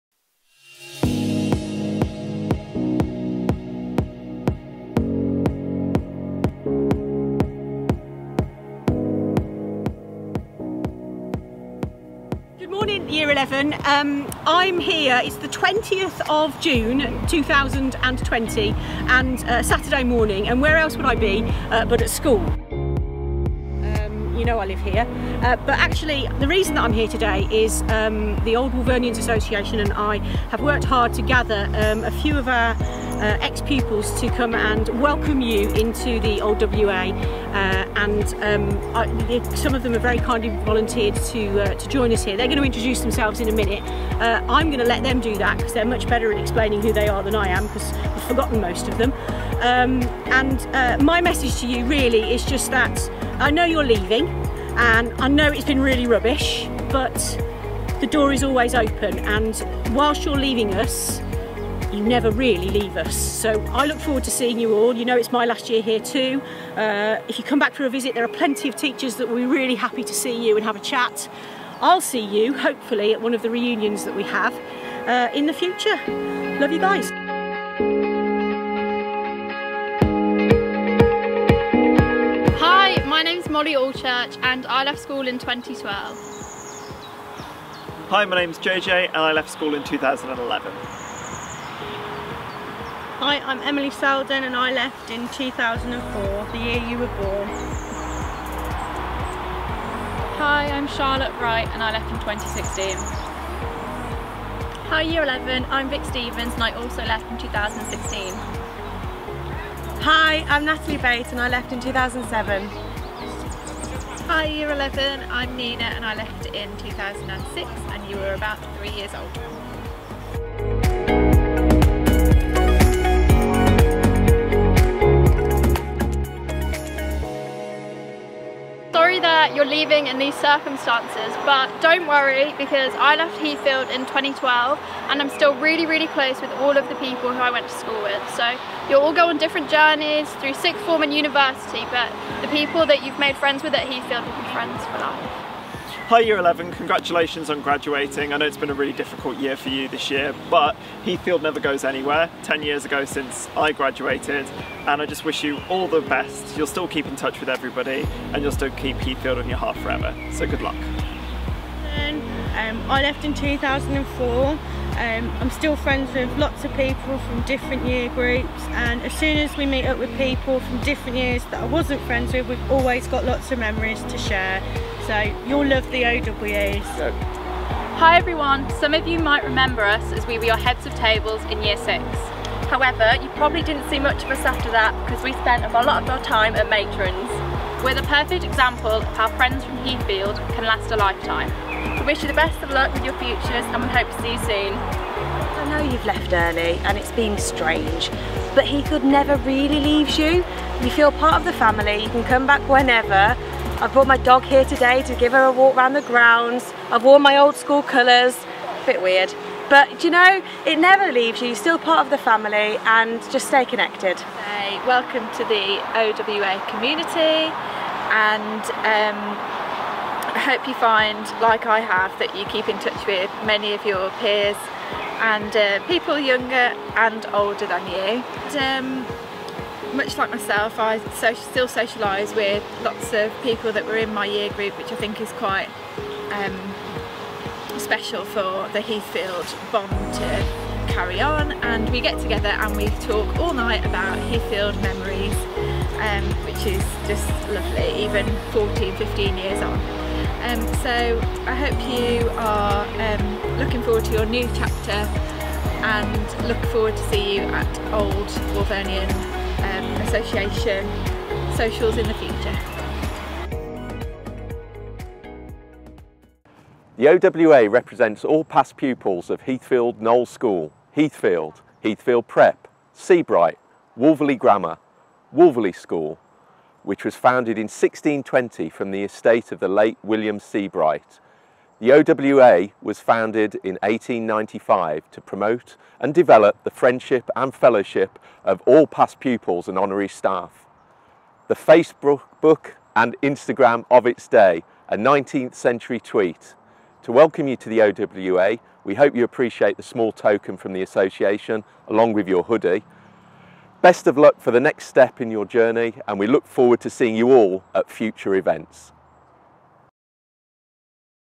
Old Wolvernian Association introduction
Situated in the picturesque village of Wolverley, within the heart of Worcestershire. Heathfield Knoll School and First Steps Day Nursery is an independent school for pupils aged 3 months to 16 years which embodies traditional heritage and ethos with a modern approach.